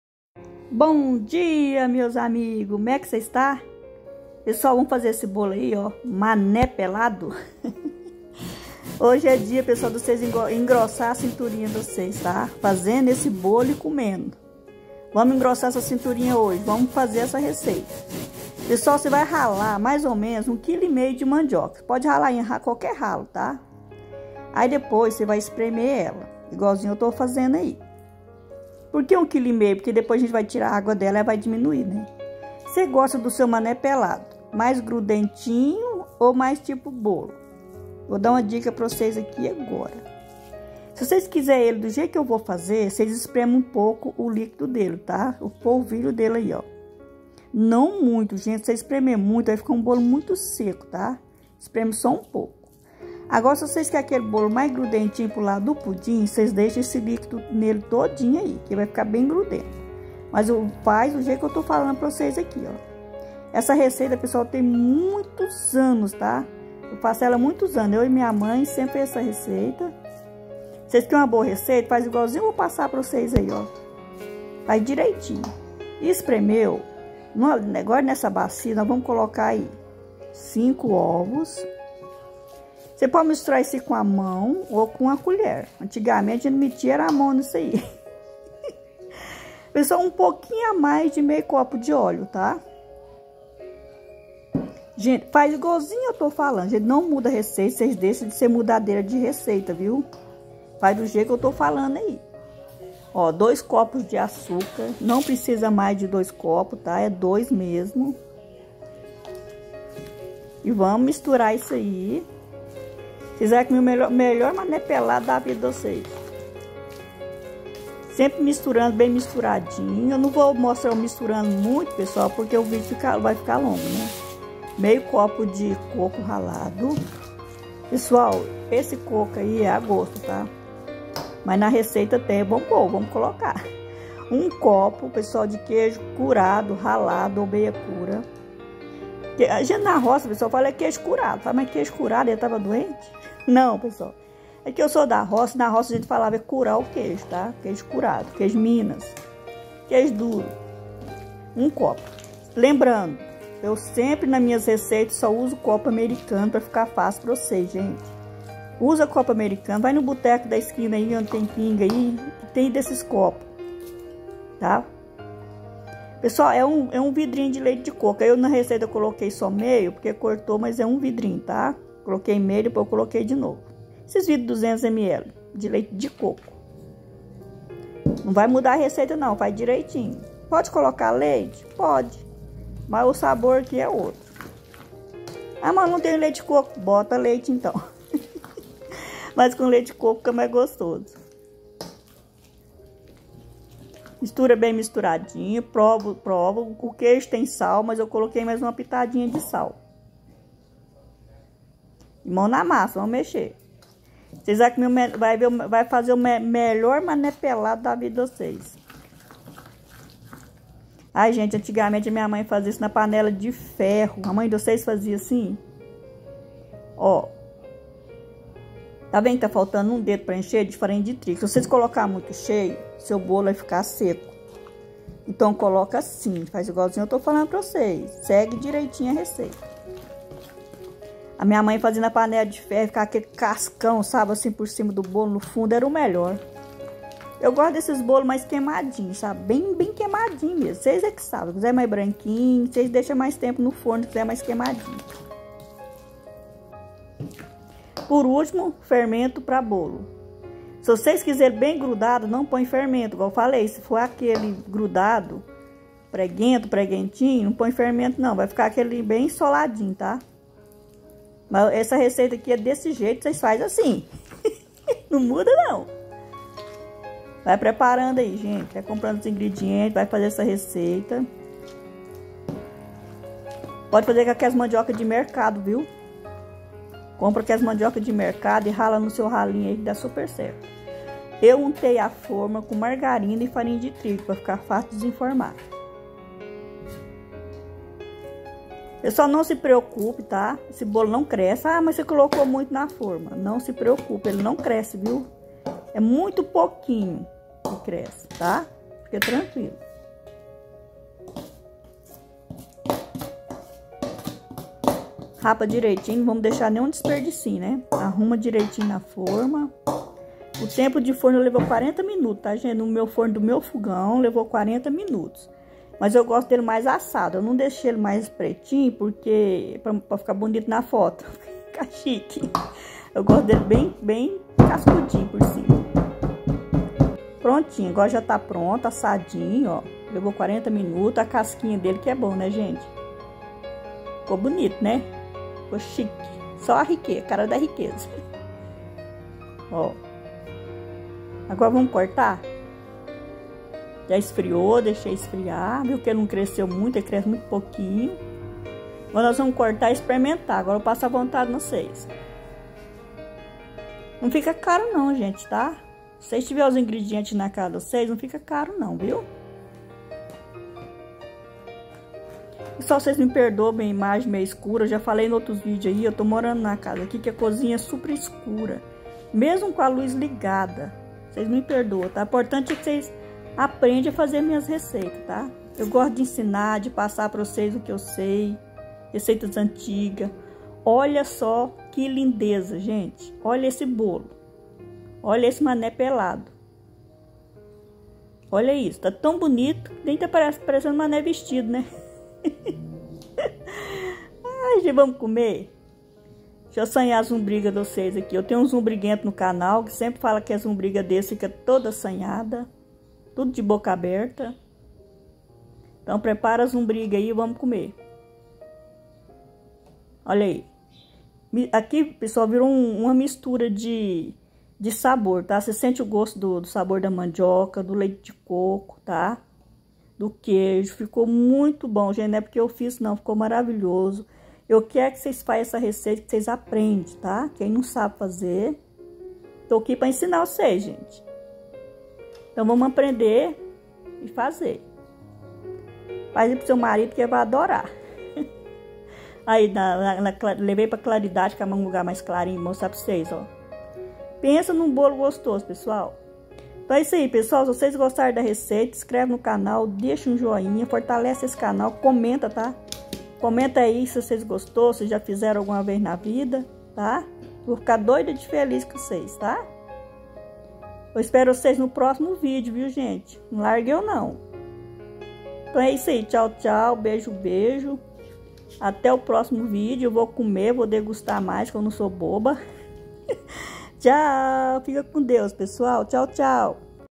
COMO FAZER MANÉ PELADO OU BOLO DE MANDIOCA .RECEITA RÁPIDA E FÁCIL.
Olá meus trem Bao..

vamos aprender mais uma receita.
    MANE PELADO.
01 KG DE MANDIOCA ESPREMIDA
05 OVOS
02 COPO DE AÇÚCAR
01 COPO DE QUEIJO
MEIO COPO DE ÓLEO
01 VIDRO DE LEITE DE COCO
MEIO COPO DE COCÔ RALADO
01 COPO DE FARINHA DE TRIGO. FALTANDO UM DEDO PARA ENCHER.
01 COLHER DE FERMENTO PARA BOLO.
 VEJA O VIDIO TODO E FAÇA IGUAL ESTA NO VIDIO.VOCES VAO GOSTAR MUITO DESSA RECEITA.

FORMA E 33 CM.
ASSOU EM 40 MT NO FORNO DE FOGAO A GÁS.

OBRIGADA A TODOS POR ESCREVER NO CANAL.

DEUS ABENÇOE VC E SUA FAMÍLIA.

SEJAM UMA BENÇÃO.